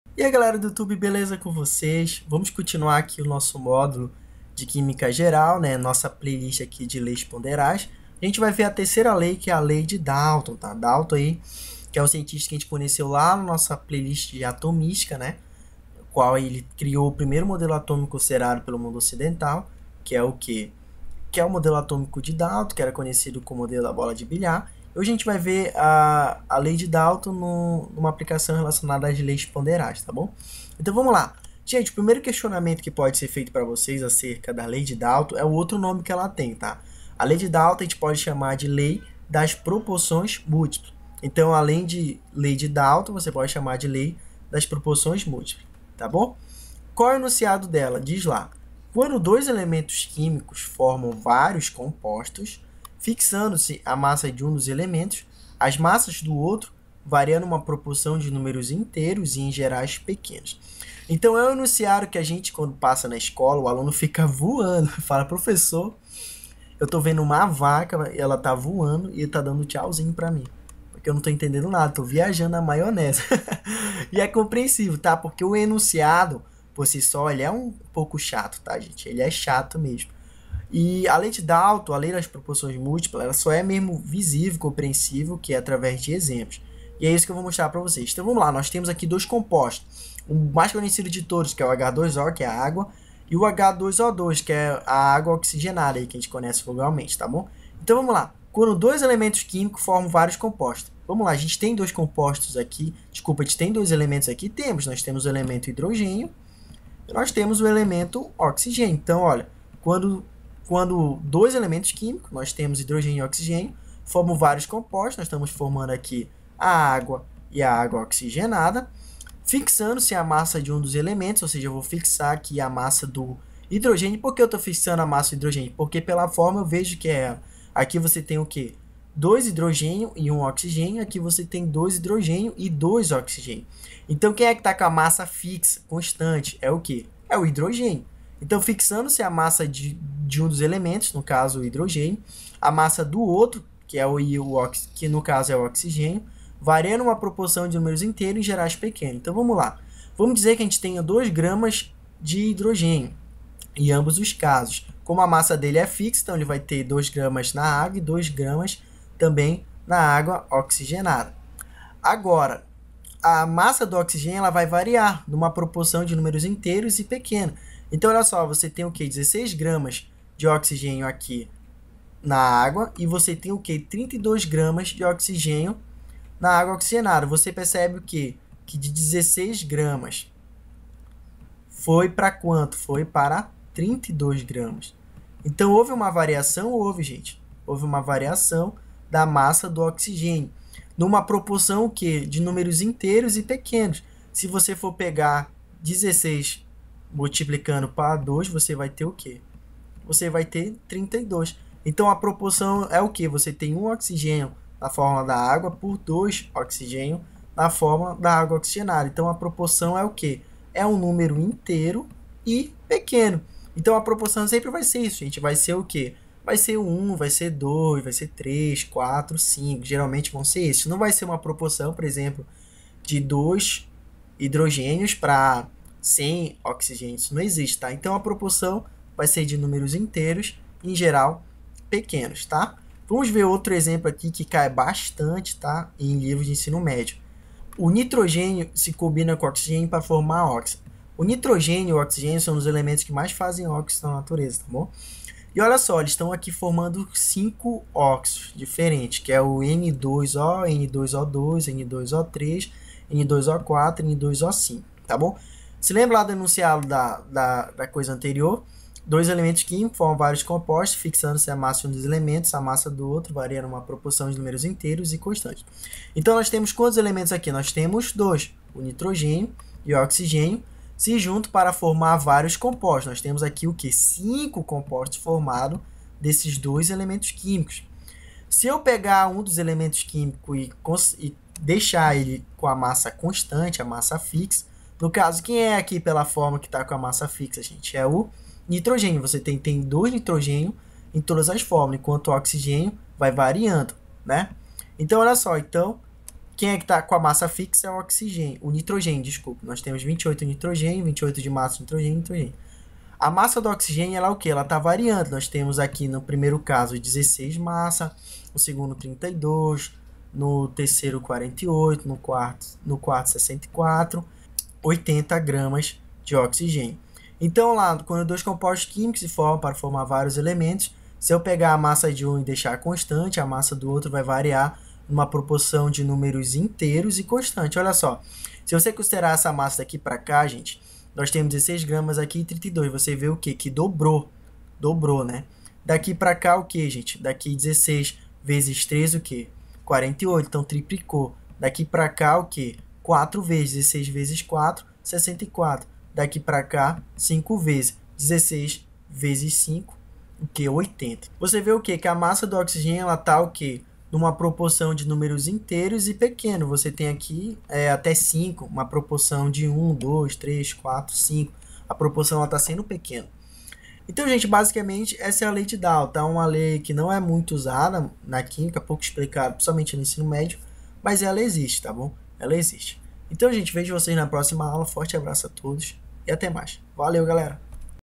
03 - Lei de Dalton
Nesse vídeo veremos a lei de Dalton, também conhecida como lei das proporção múltiplas.

Contribua com canal para adquirimos um cenário para gravação dos vídeos:
Agência: 0001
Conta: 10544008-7
Tipo de pessoa: Física